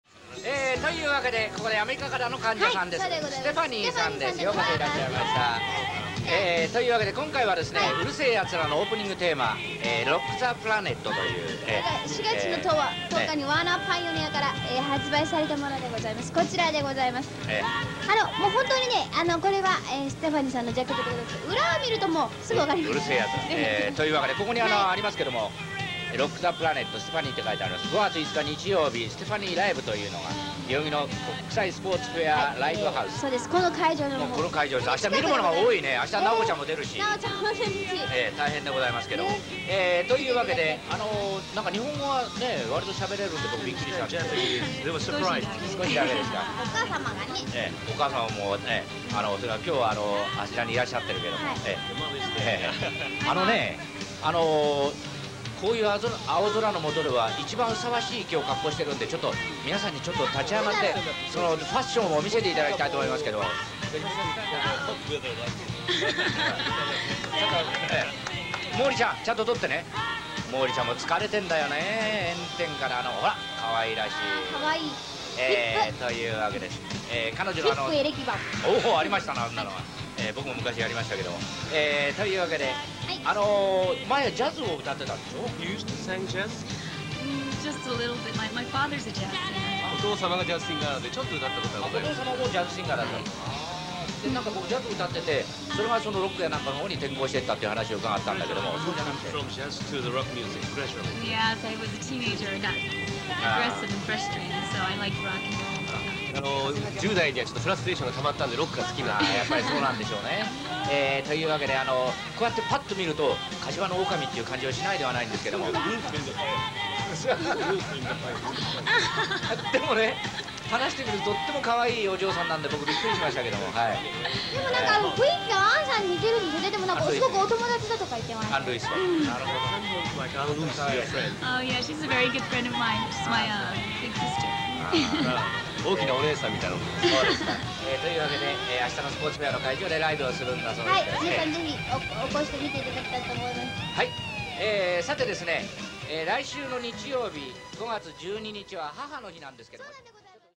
Steffanie interview at Yoyogi Stadium
Rare TV show interview... Steffanie being interviewed at Yoyogi Stadium for a Boys/Childrens Day event (pre-show interview before performing live on the TV show).